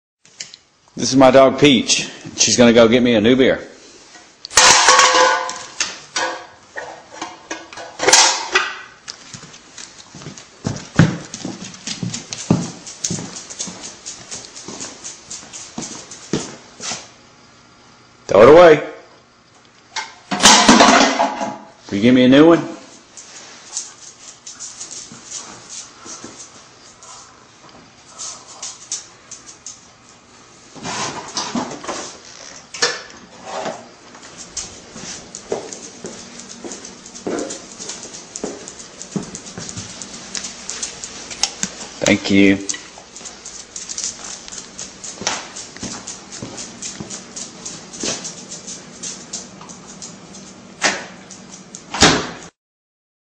Haal eens een biertje
Deze hond kan een biertje halen erg handig.